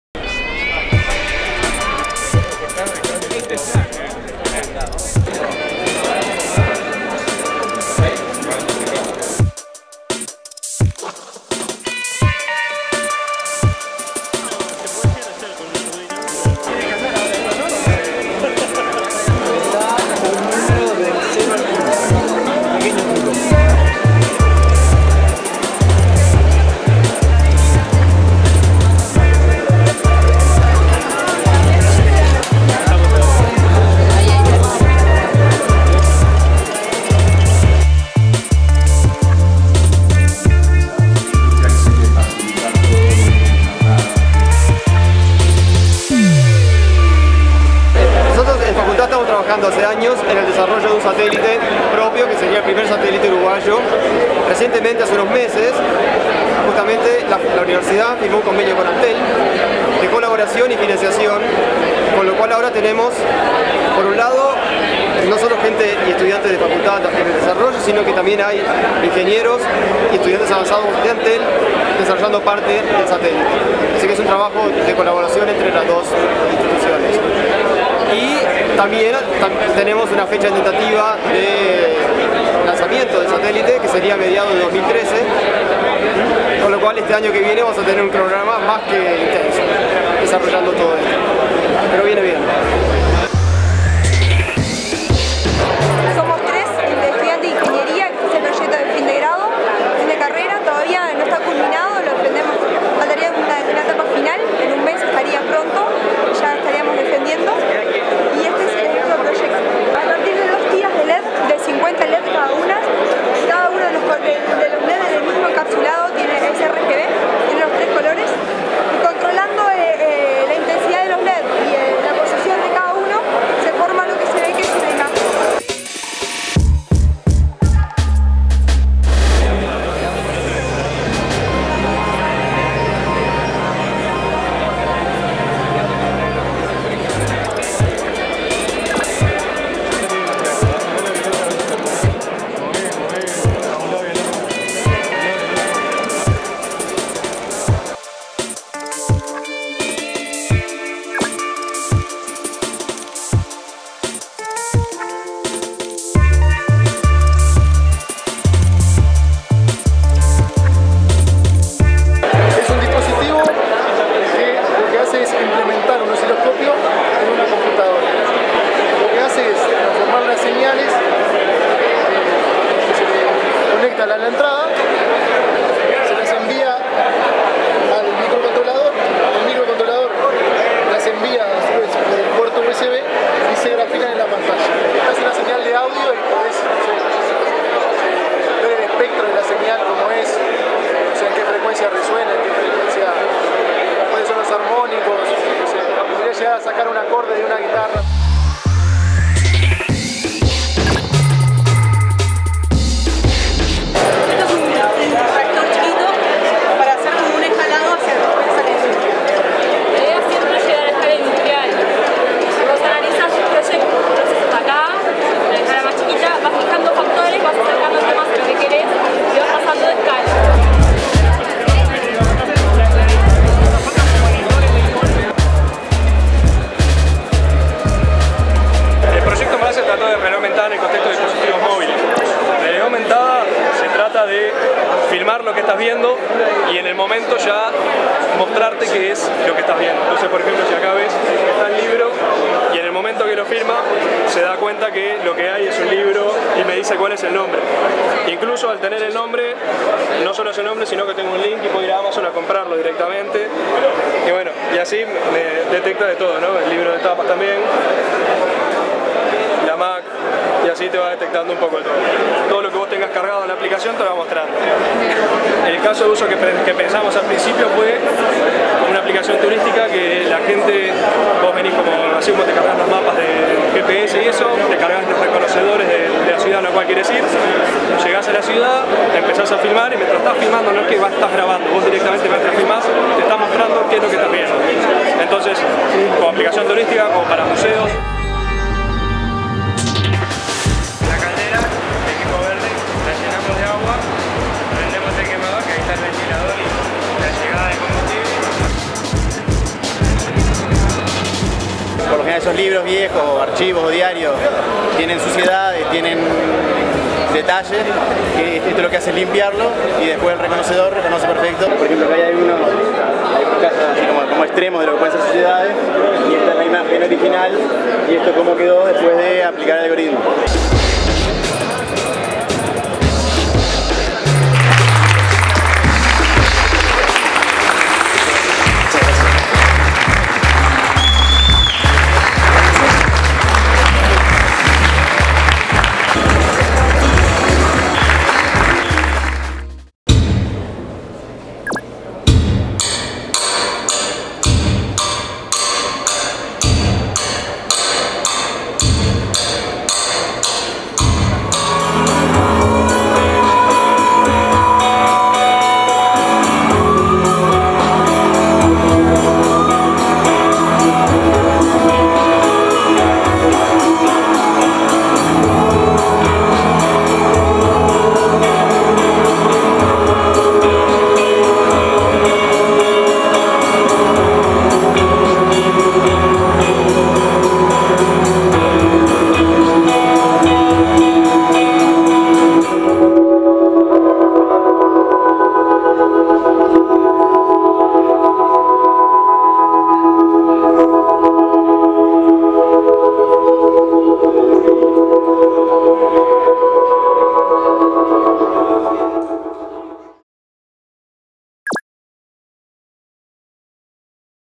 Ingeniería deMuestra 3
Ingeniería deMuestra es una jornada organizada por la Facultad de Ingeniería y su Fundación Ricaldoni, que se desarrolla desde hace tres años con el objetivo de mostrar al público los trabajos que generan los estudiantes de grado y de posgrado para solucionar problemas prácticos que existen en el país. También se exponen otros trabajos de investigación y de emprendedores de la Fundación Ricaldoni.

Entre 106 prototipos, proyectos y desarrollos innovadores presentados, los visitantes tuvieron la oportunidad de observar robots, aplicaciones de realidad aumentada y Google Earth, software para fútbol, y el análisis comparativo entre la patada de un nadador y el nado de los peces.

Facultad de Ingeniería, 10 de noviembre de 2011.